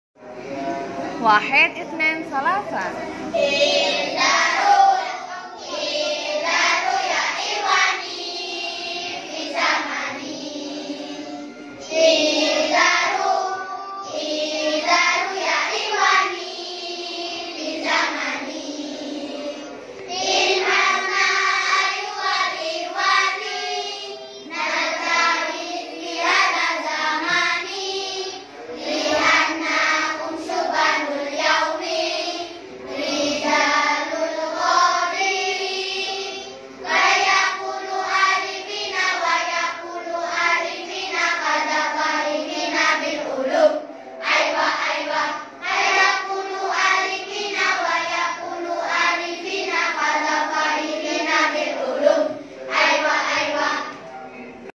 Kini Agen, Kelak Pelopor : Moderasi Beragama
14. Dewasa ini, Moderasi Beragama secara masif digaungkan oleh Kementerian Agama Republik Indonesia dan Moderasi Beragama menjadi salah satu Pendekatan dalam Pembelajaran Agama di seluruh tingkatan pendidikan, baik tingkat dasar, menengah, atas, maupun tingkat tinggi.

Lagu Berbahasa Arab ini mengandung makna yang dalam mengenai Siswa-Siswi sekarang yang akan menjadi pemimpin di masa depan. Apa yang kita tabur akan kita tuai. Menyiapkan peserta didik yang moderat, melahirkan pendidik-pendidik dan generasi yang moderat, damai, dan bijaksana serta benar² menerapkan Islam 'Rahmatan Lil Alamin'.

This was taken on March, 05th, 2020 at 07.56 a.m